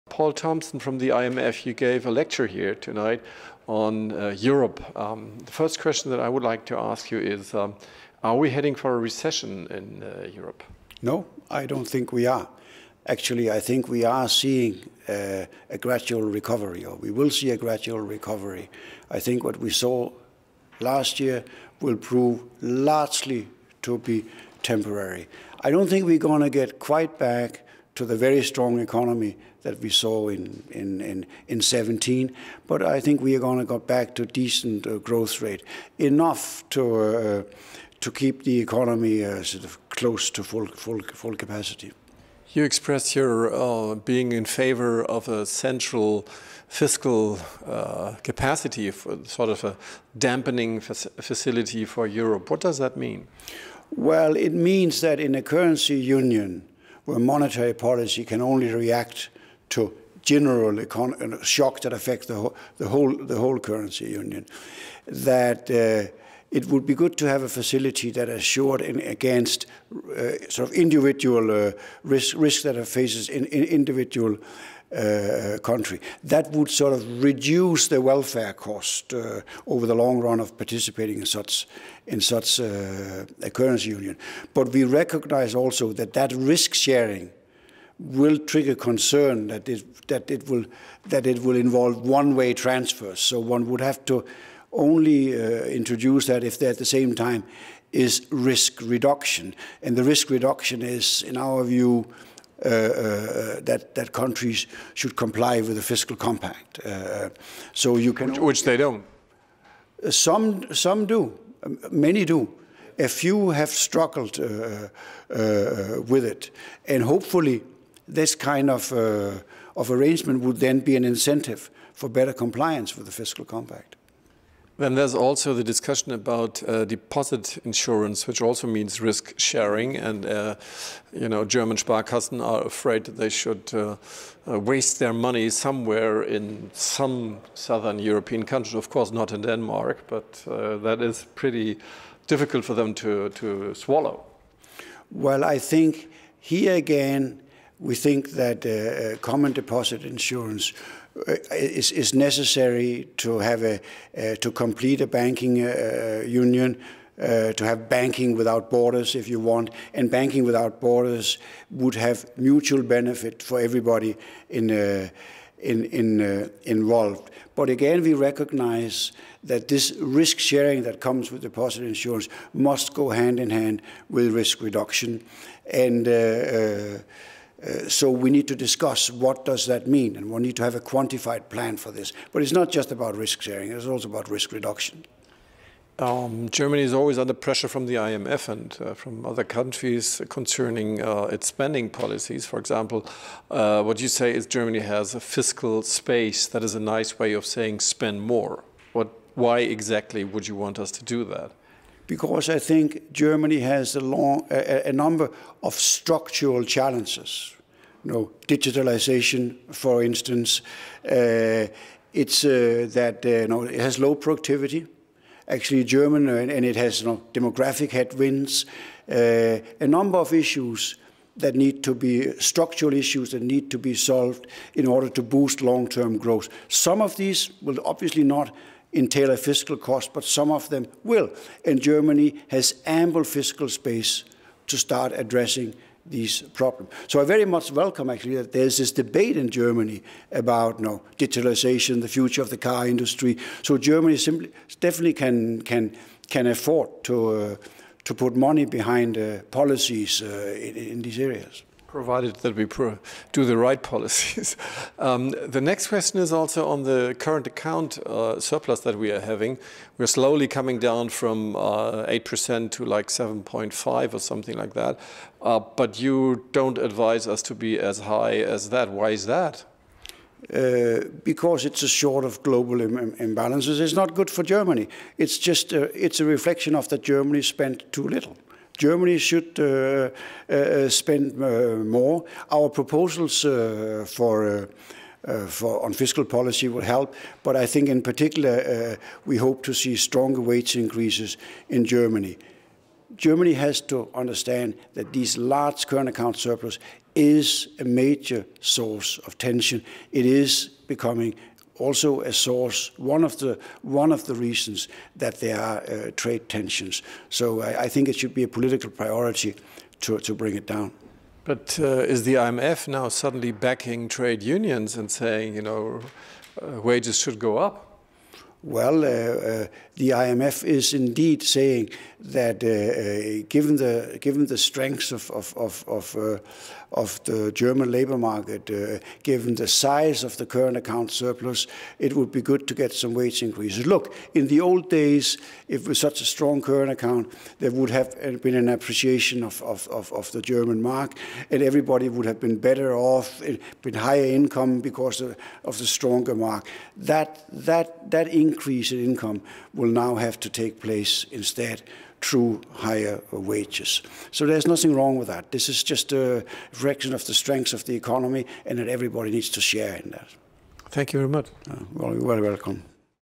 Interview on The Euro Zone and Germany: Challenges and Risks
Munich Seminar on 13 May 2019 with Poul M. Thomsen, Director, European Department, International Monetary Fund (IMF)

ifo Institut – Leibniz-Institut für Wirtschaftsforschung an der Universität München e.V.